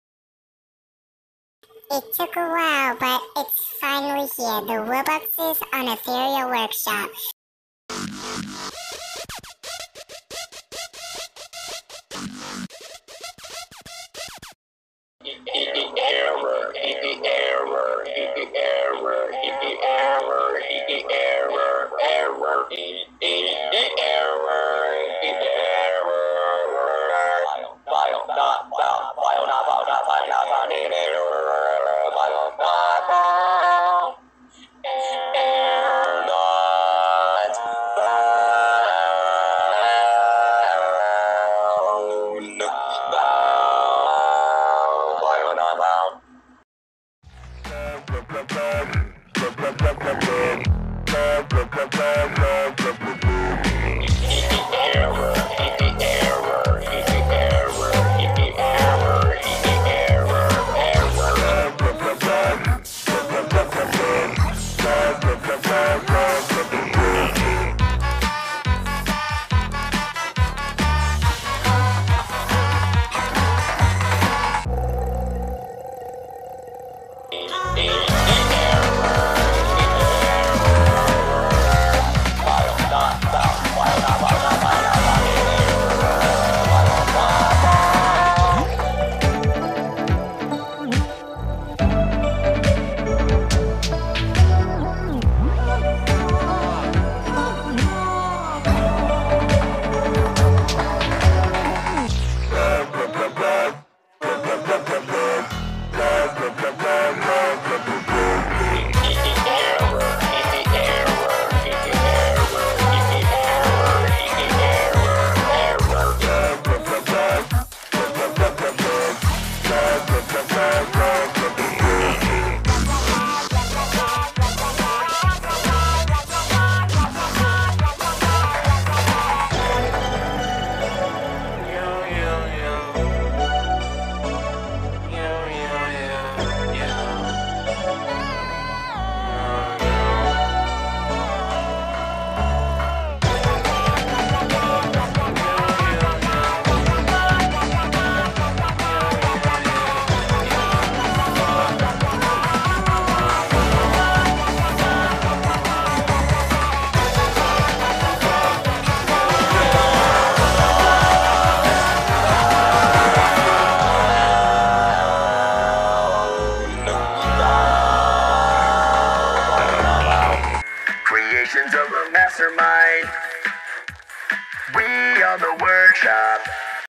Wubboxes on Ethereal WORKSHOP! (Epic Coming Soon!)
The Rare Wubbox had better Vocals! and the Common Wubbox heard it, so he wanted to JOIN :D

Wubboxes sounds made by Me :D